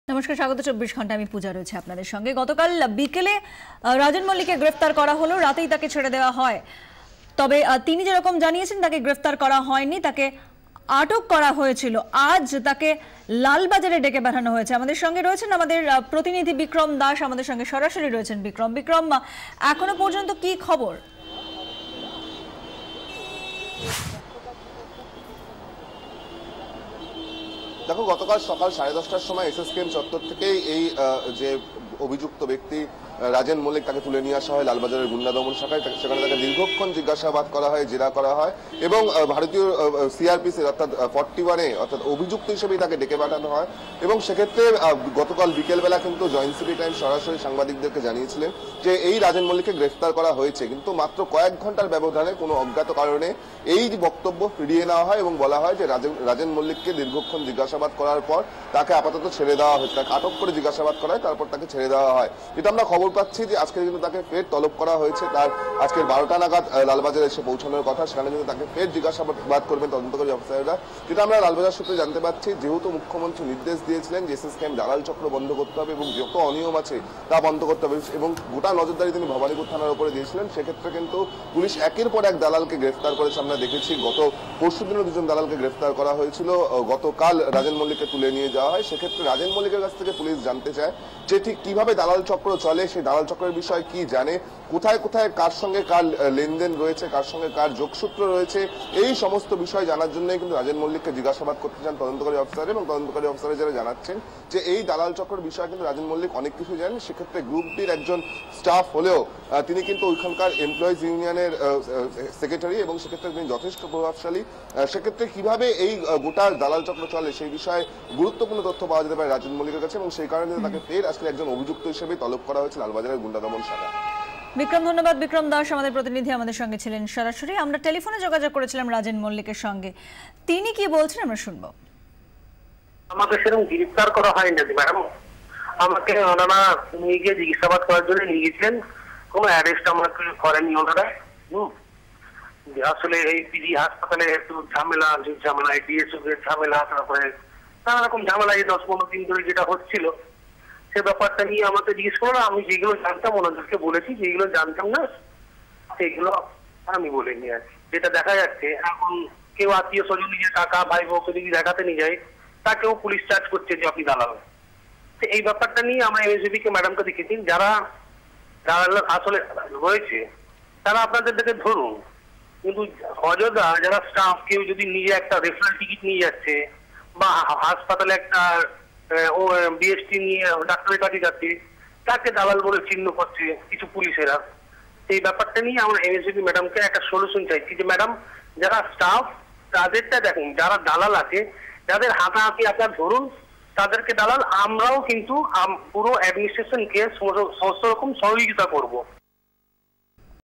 SSKM Hospital tout racket Kingpin Rajan Malik released after his arrest. Says, he was not arrested
SSKM Hospital tout racket Kingpin Rajan Malik released after his arrest, Says, he was not arrested. To satisfy the demands of high-rise favourites of Bengali audiences, Zee 24 Ghanta brings all the latest headlines and news stories.